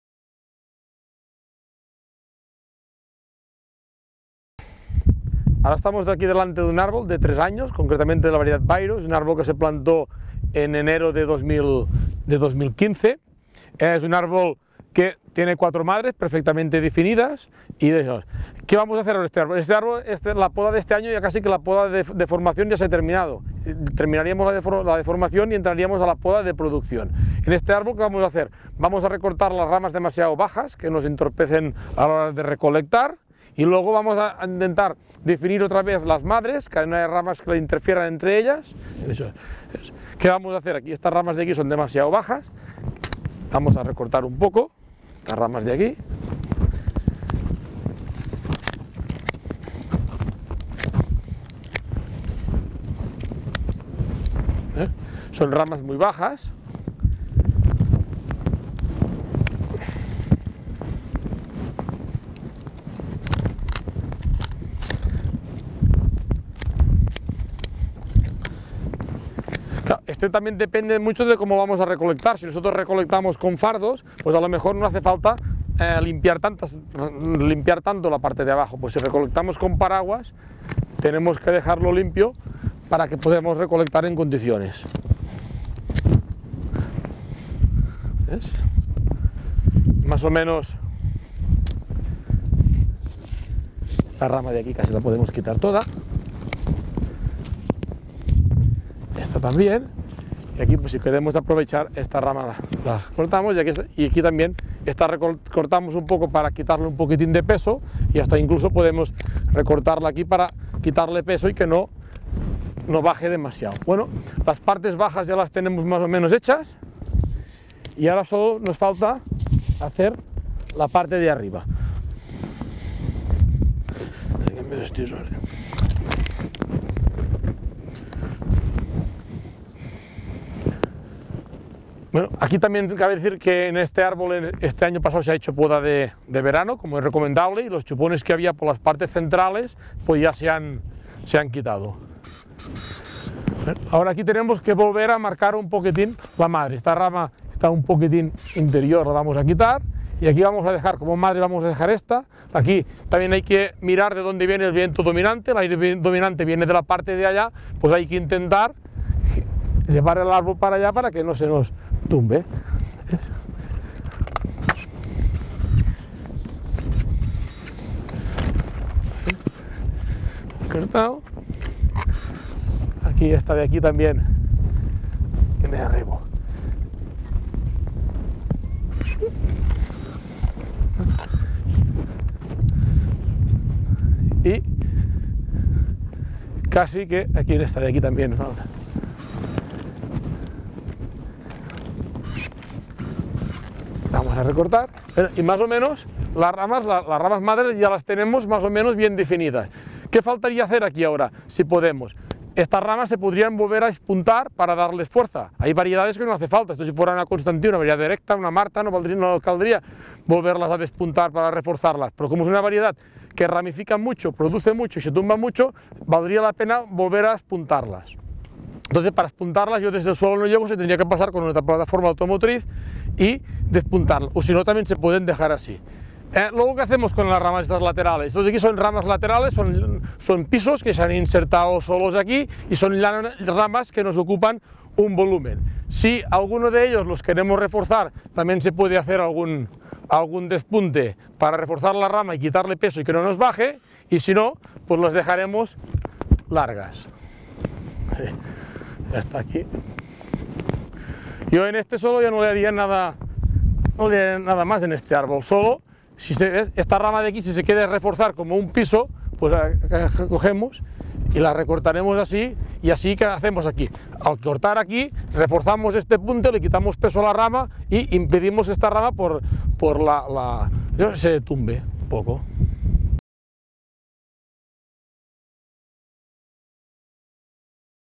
Poda de formación en plantaciones de almendro de regadío (Año 3)
El técnico del IRTA Ramon Girabet nos explica en este vídeo la poda de formación en plantaciones de almendro de regadío, en su tercer año.

Vídeo perteneciente a la formación en poda de almendro del IRTA, el Instituto de Investigación y Tecnología Agroalimentarias de la Generalitat de Catalunya. Visita nuestra web